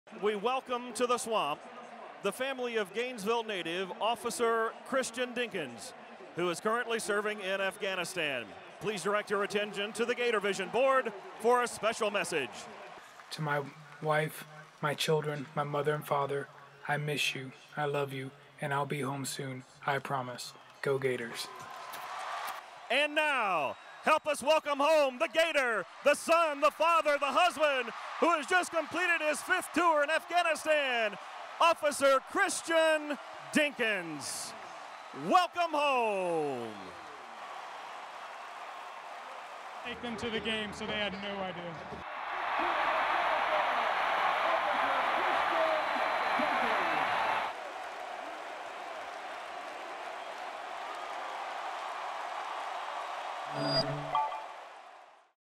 U.S. Army officer surprises family at Florida Gators game
This family thought they were going to see a recorded message from their deployed service member... when this happened.